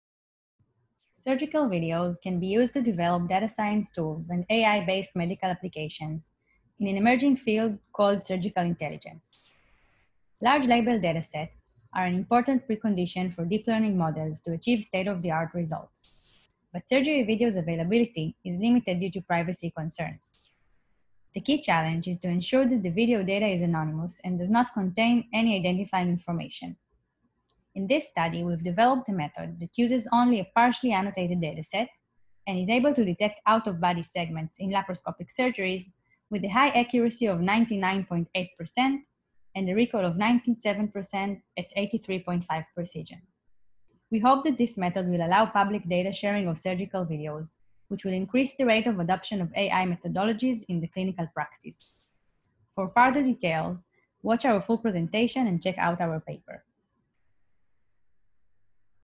MIDL 2020, P157, Zohar et al. Teaser
P157 - Accurate Detection of Out of Body Segments in Surgical Video using Semi-Supervised Learning

Maya Zohar, Omri Bar, Daniel Neimark, Gregory D. Hager, Dotan Asselmann